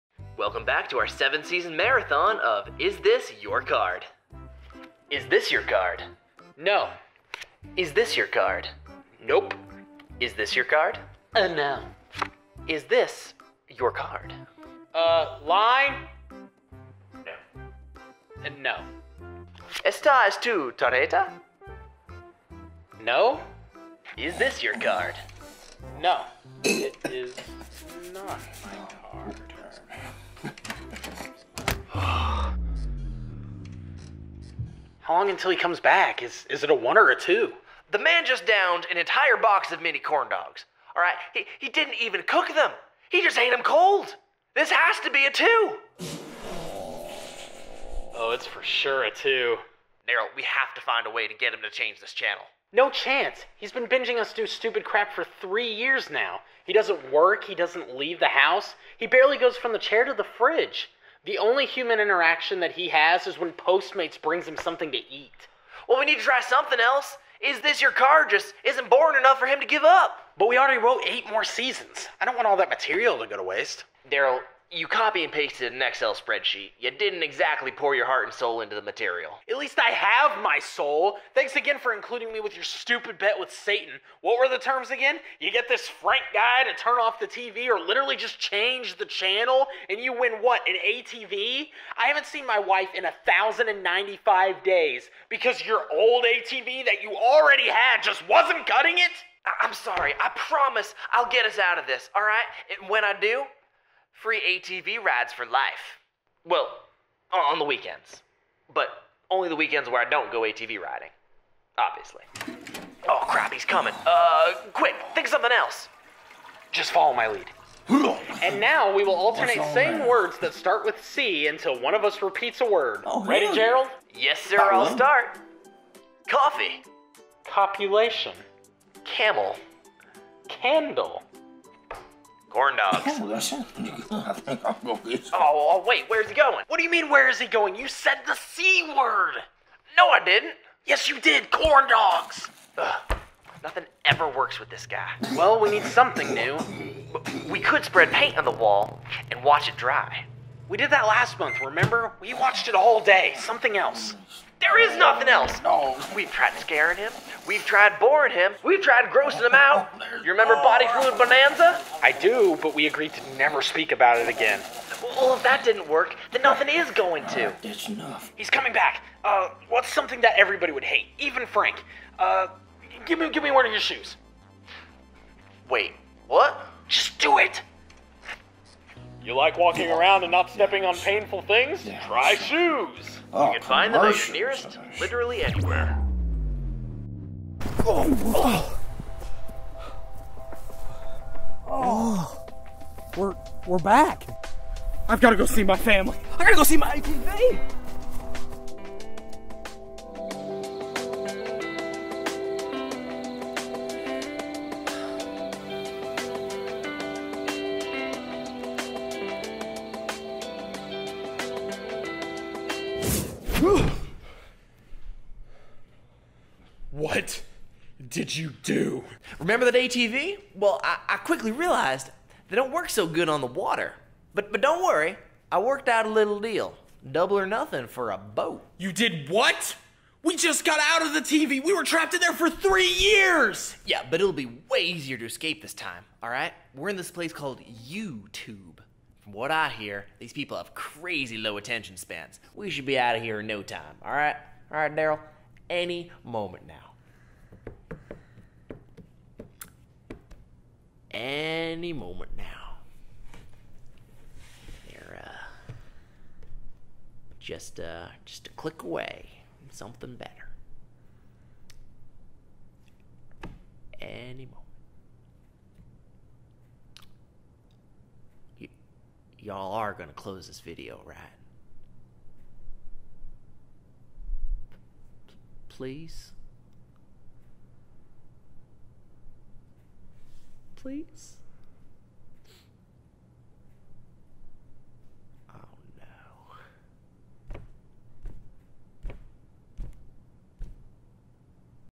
TV Purgatory
Don't ever make a deal with the devil for a new ATV, otherwise you might get trapped in the television for 3 years. Sound oddly specific? It's all too familiar for these two boys...

We're back with another sketch, I know it's been quite some time since we came out with wholly original comedy, so please let us know what you think in the comments below!